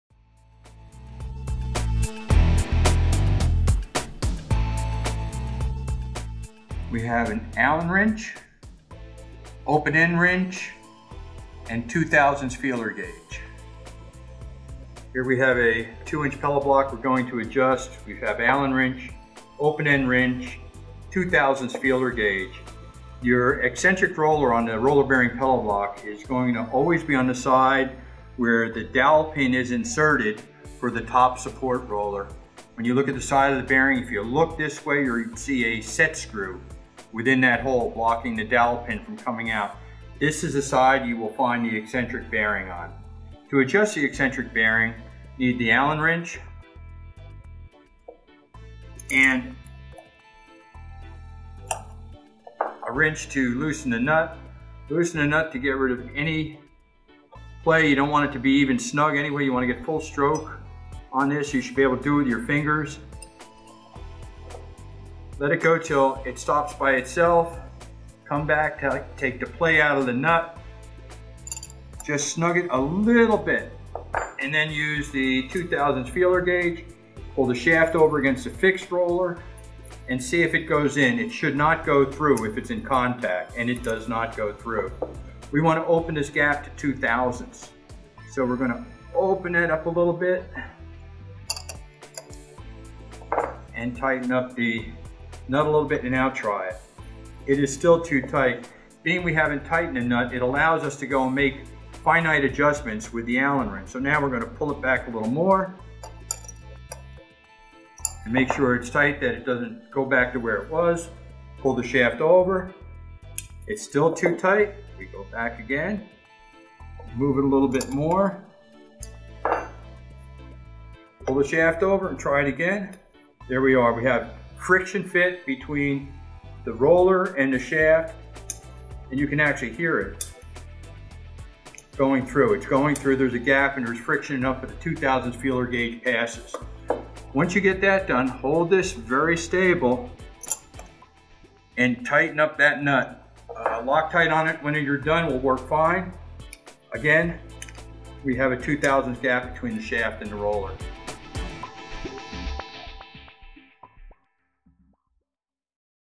How to Adjust a Roller Pillow Block
PBC Linear provides a hands-on demonstration of how to adjust the eccentric bearing on a Roller Pillow Block system. The Roller Pillow Block provides key performance benefits such as high load capacities, high speeds, and design compatibility—providing size interchangeability with industry-standard block.